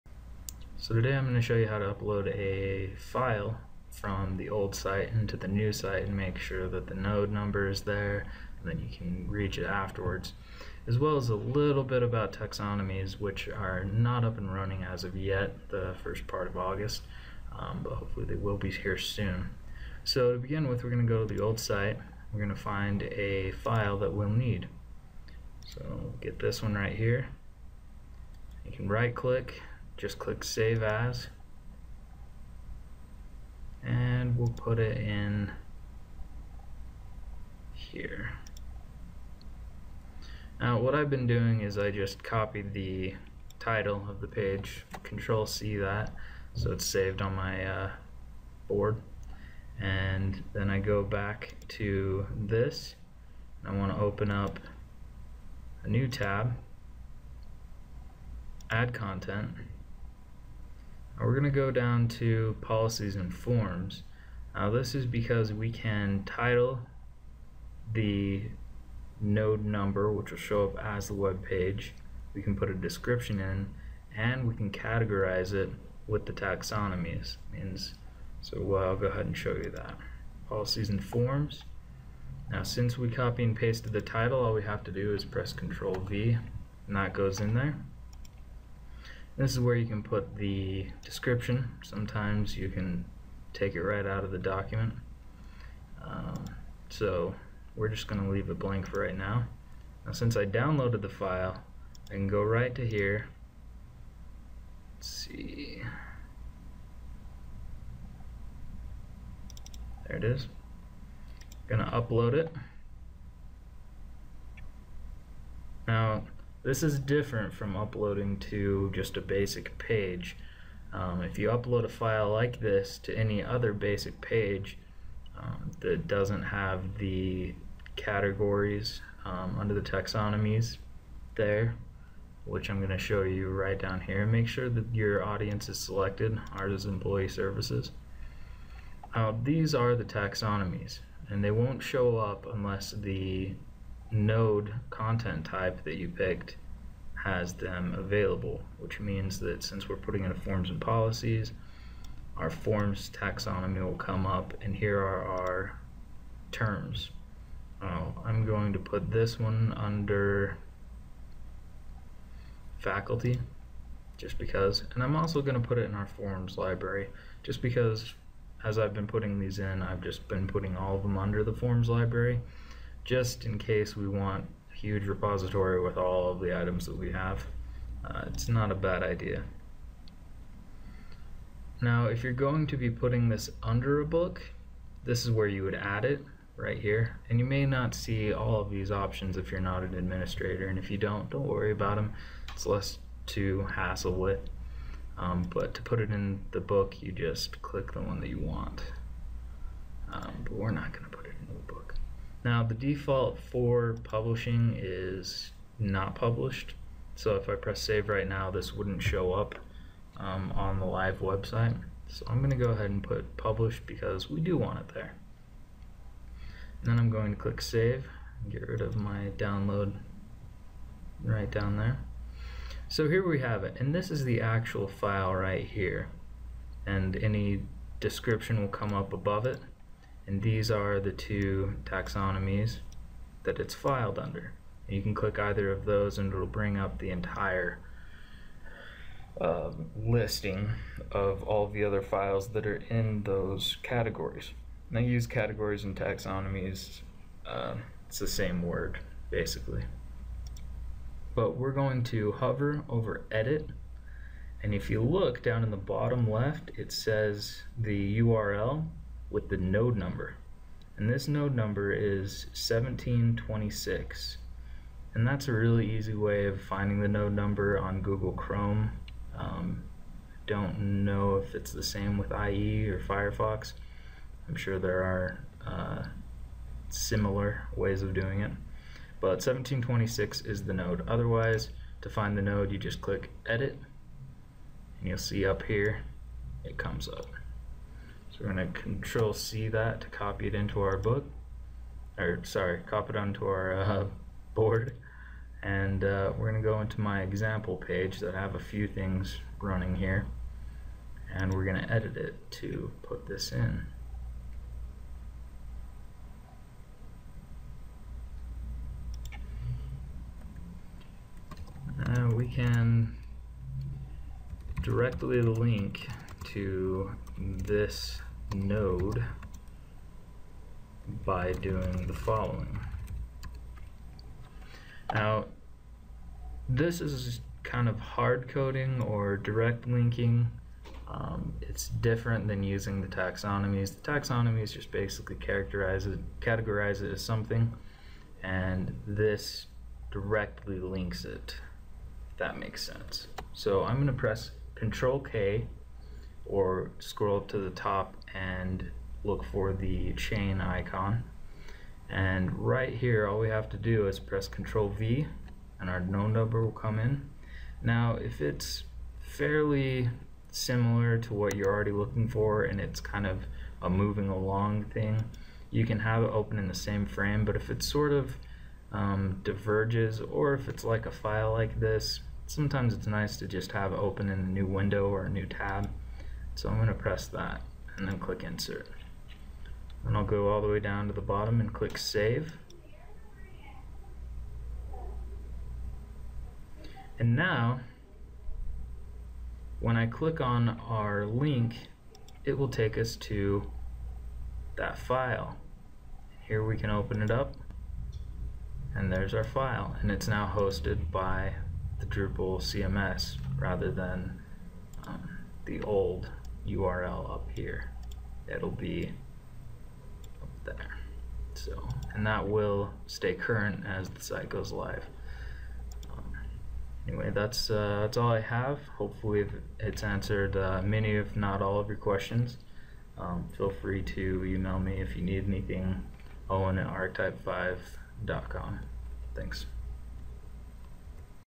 Add a File and Link to it (Content in Drupal)
Find out how to add a file and link to it in the Drupal CMS. This is part of a series of videos on adding content to your Drupal site.